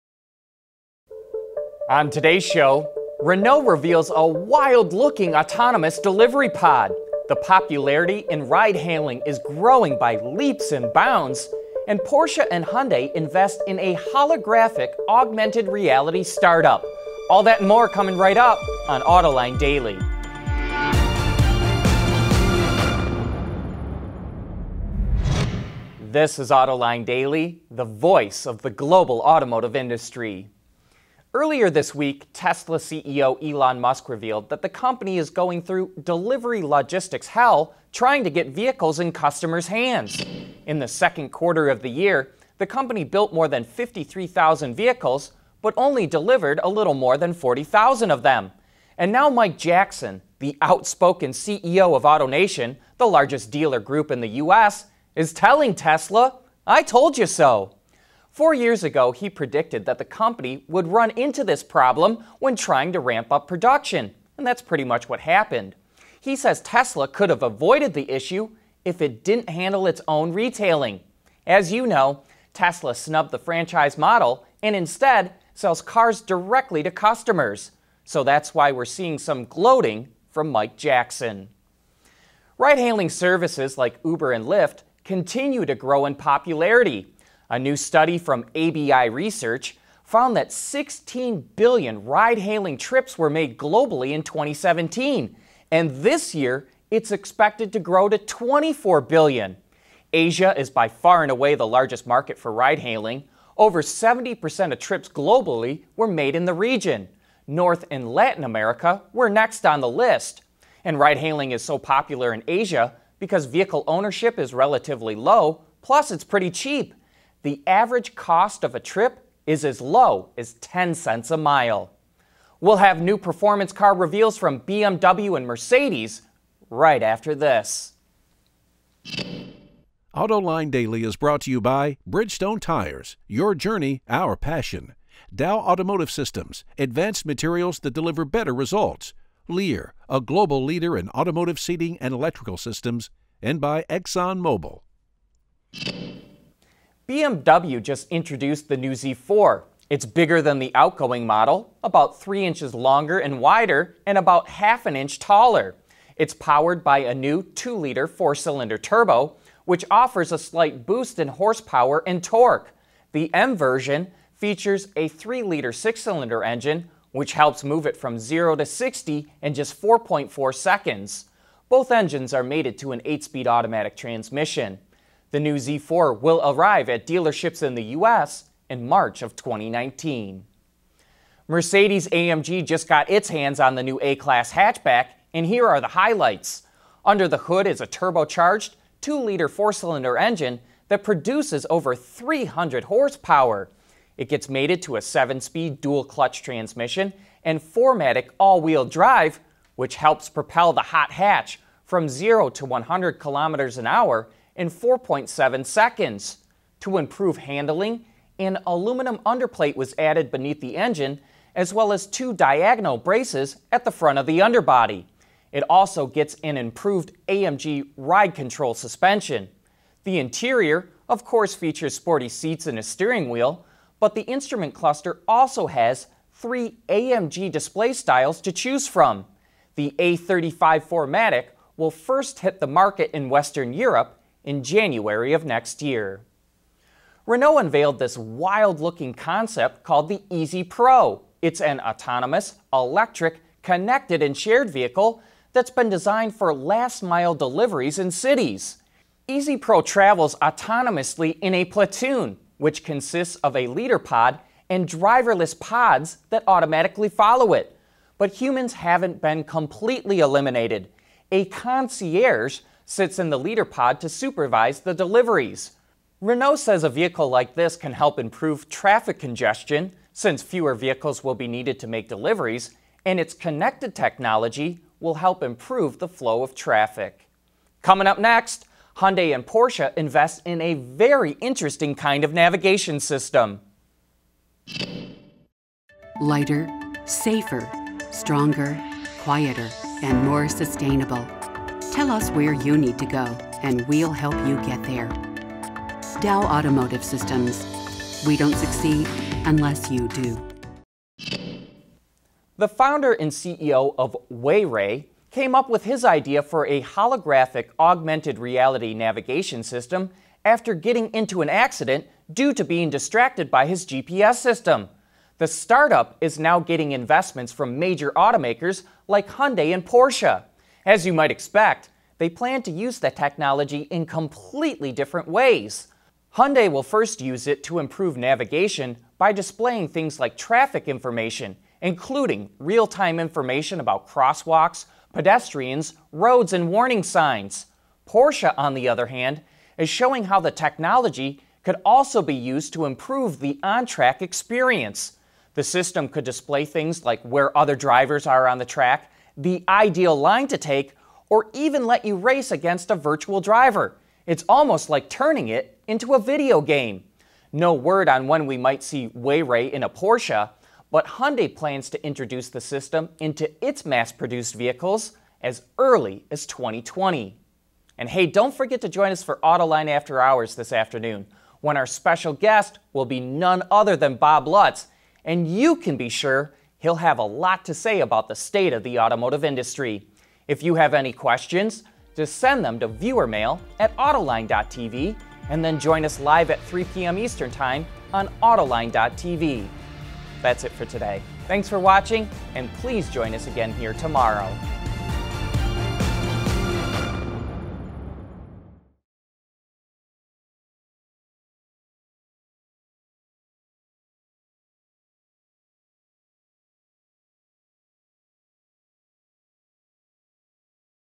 Wild Nav System, New AMG A-Class - Autoline Daily 2439
0:31 AutoNation CEO Tells Tesla 'I Told You So'
1:22 Ride-Hailing To Grow Significantly in 2018
2:26 BMW Reveals New Z4
3:01 AMG Gets Its Hands on New A-Class
3:57 Renault Develops Last-Mile Pod Concept
5:02 Hyundai & Porsche Invest in Wild Nav System